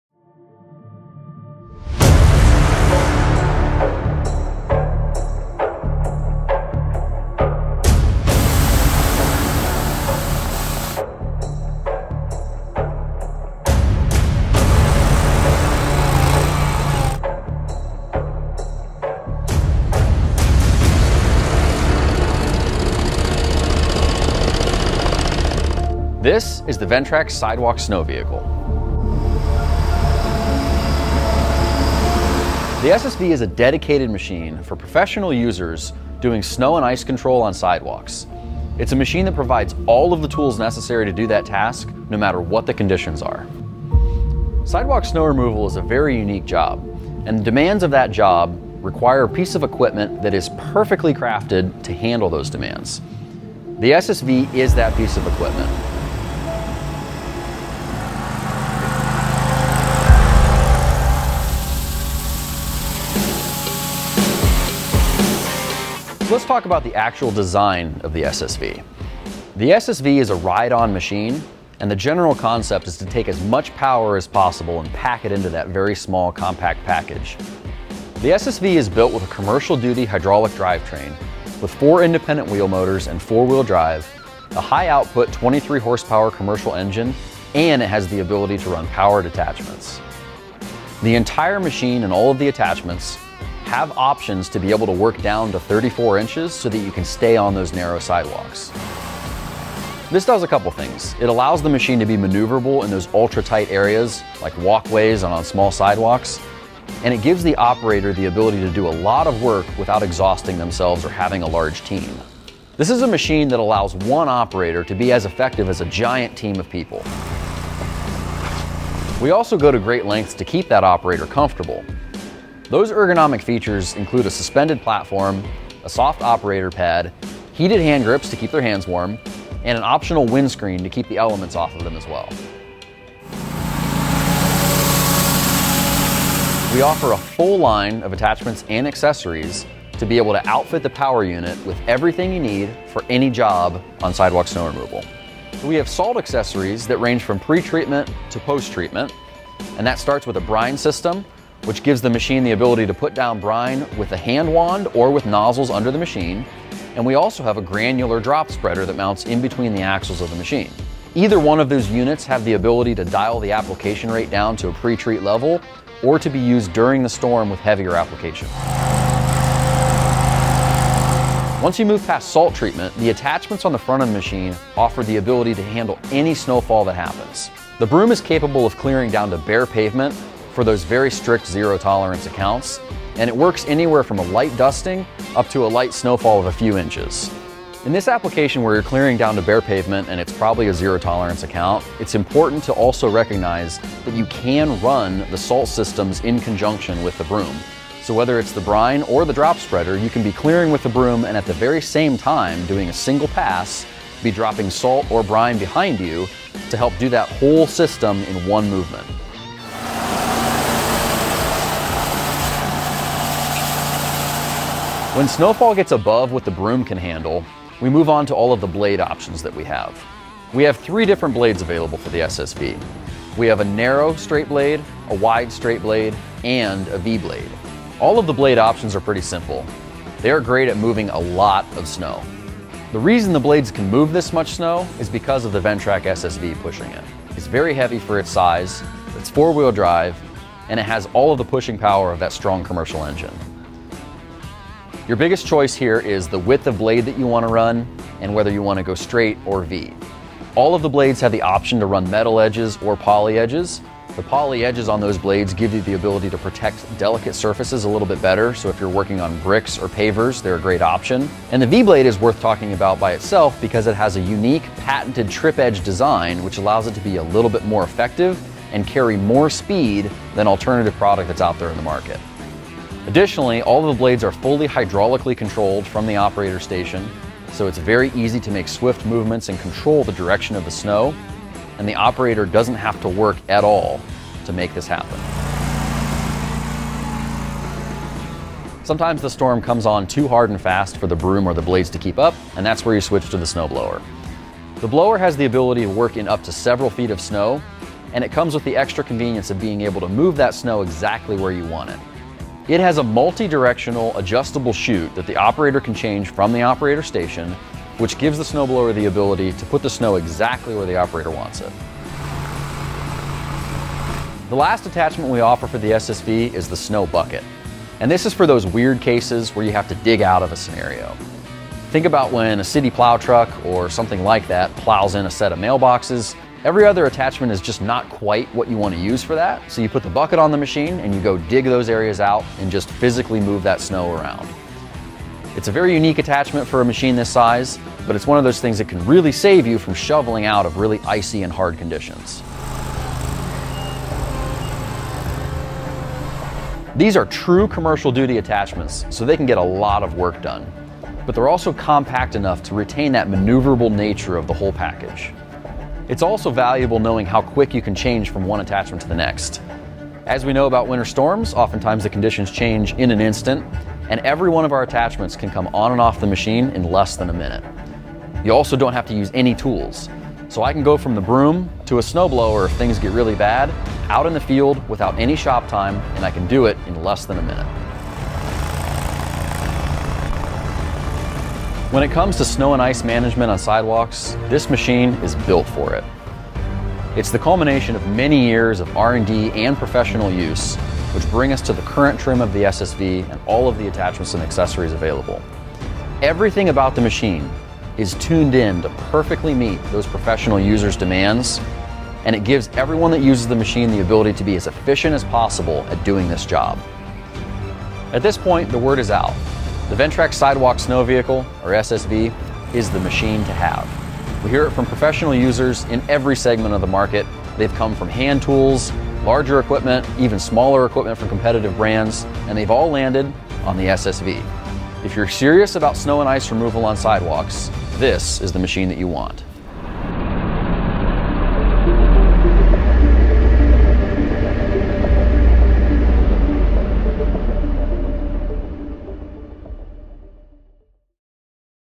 Ventrac SSV (Sidewalk Snow Vehicle) | Zero-Tolerance Sidewalk Snow Management
The Ventrac SSV is your one-pass snow removal and de-icing machine, engineered to be a system that can handle any winter storm. The Sidewalk Snow Vehicle is a refined and durable option for those zero-tolerance accounts, making it easy to manage snow and ice on sidewalks when you need it most.

The SSV allows for further reduction of hand labor and walk-behind equipment than ever before. With stand-on 4x4 skid steer operation, the SSV has a 36” working width that allows you to travel miles of sidewalk in a fraction of the time, furthering the reduction of hand labor and walk-behind equipment. With a full line-up of commercial-quality snow removal attachments and de-icing options, the SSV is sure to be a game-changer for you this winter season.

A SIDEWALK SNOW VEHICLE BUILT FOR PROFESSIONALS
The Ventrac SSV is setting the standard for sidewalk snow management. With multiple attachments and a 34" machine width, the SSV is ready to access the tightest areas of any property, making sidewalk snow management easy and efficient!